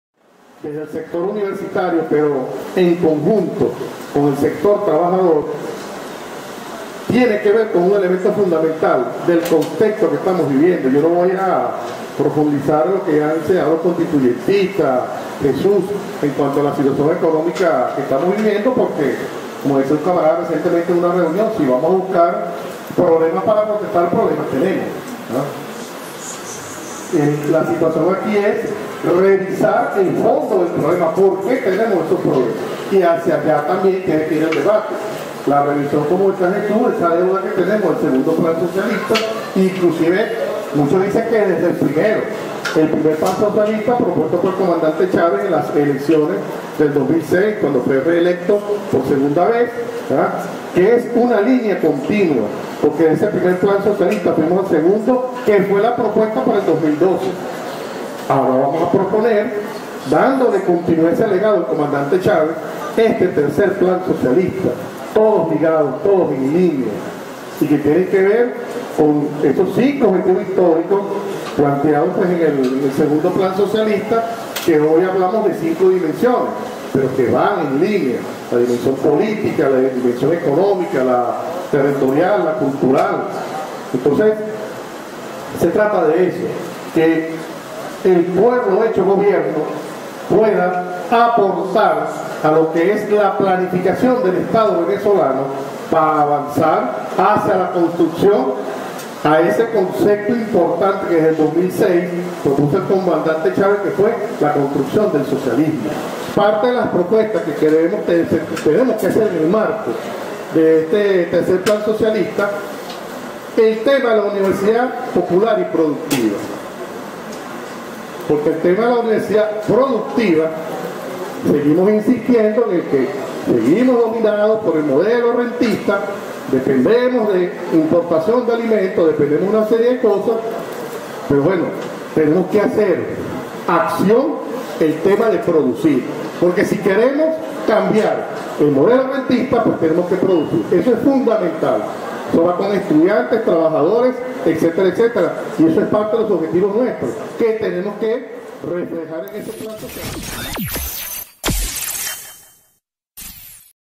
UNIVERSITARIOS Y EL PLAN DE LA PATRIA
Debate iniciado en los espacios de la UPT de Yaracuy, allí instituciones educativas y demás entes adscritos al Ministerio del poder Popular para la Educación Universitaria, Ciencia y Tecnología (Mppeuct), se constituyeron en equipos de trabajo para elaborar propuestas al Plan de la Patria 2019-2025.